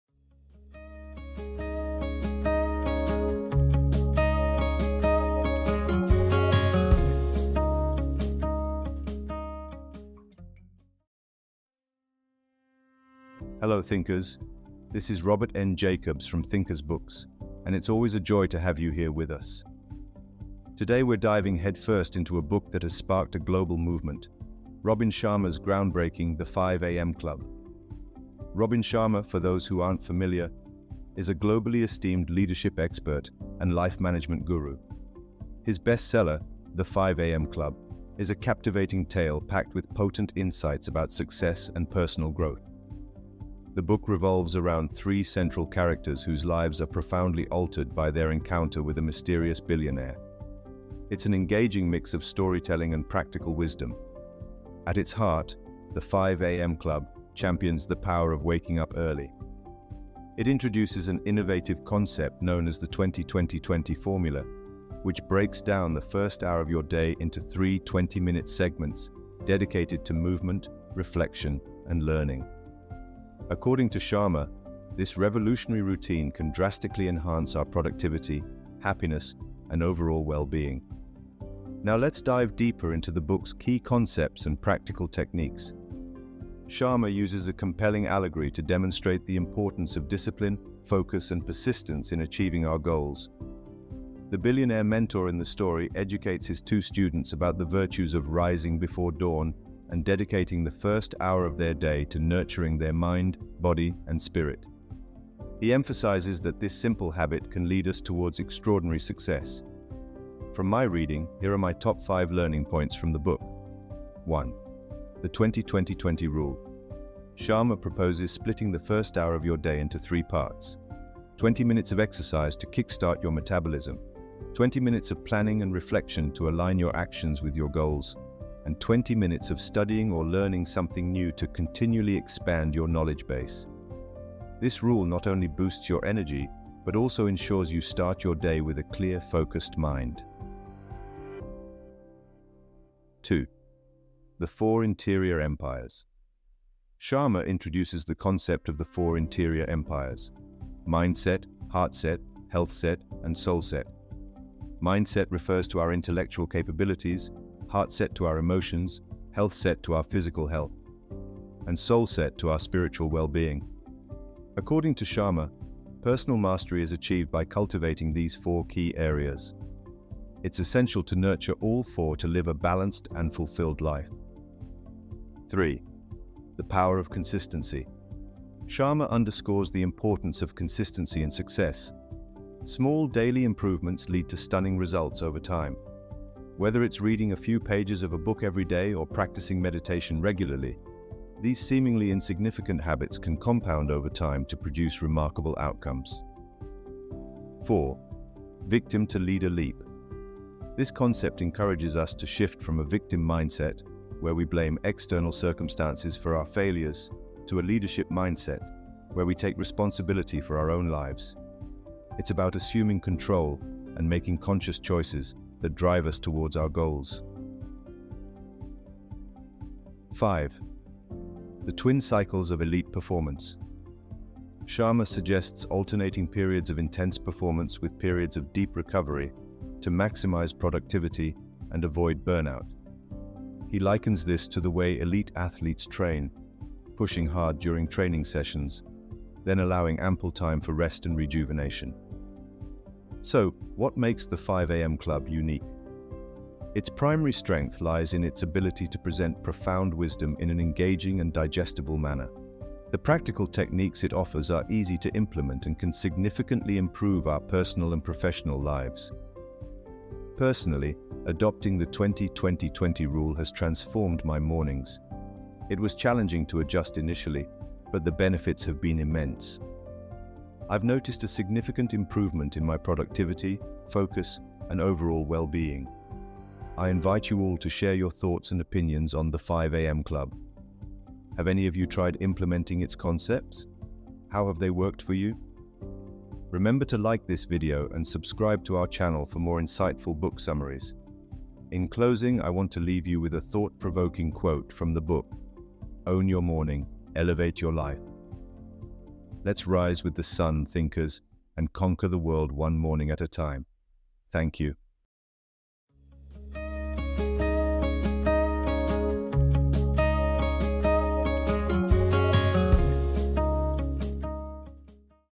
The 5AM Club | Thinkers Books
"The 5AM Club" by Robin Sharma is a self-help book that emphasizes the value of waking up at 5 AM. The book is structured as a story featuring an entrepreneur and an artist who meet a billionaire mentor. This mentor teaches them about the importance of a revolutionary morning routine to achieve personal growth and maximize their potential.

The book presents a compelling argument for a work-rest routine to join the top 5% of performers. It suggests that the first hour of your day, if used effectively, can drive personal growth and help you get the most out of life.

The key concept of the book is the 20/20/20 formula. This means dedicating the first 20 minutes after waking up to intense exercise, which helps to wake up the body and mind. The next 20 minutes are for planning and reflecting on your goals, and the last 20 minutes are spent learning something new.

However, the book also acknowledges that this routine may not suit everyone, as it could interfere with the body's natural sleep-wake cycle, particularly for those who are more energized at night.

Overall, "The 5AM Club" is about establishing an early morning routine to boost mental focus, enhance productivity, and reach peak performance.